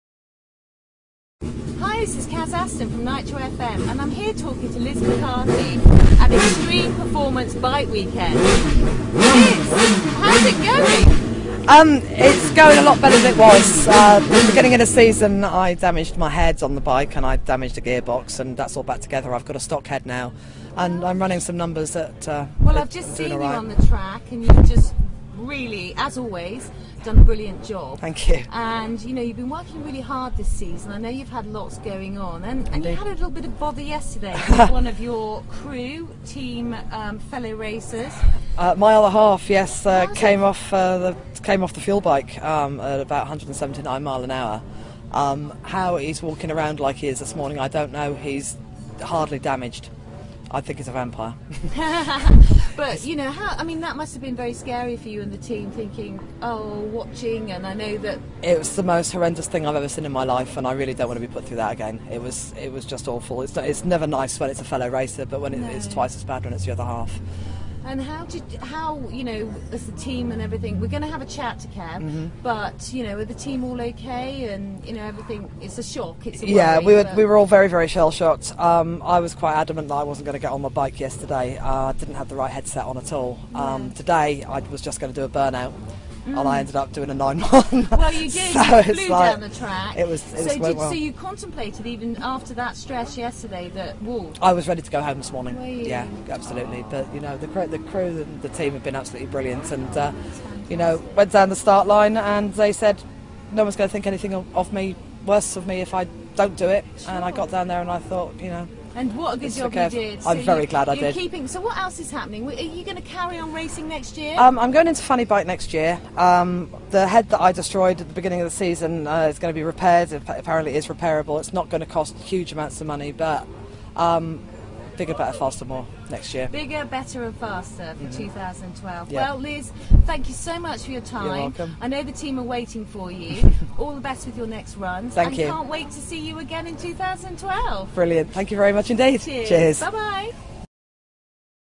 Girls Racing motorbikes would you?
Liz McCarthy a female motor bike rider talks about her 2011 racing season to Nitro FM. Liz and her team are really progressing very well and just keep getting faster! Girls can do it too.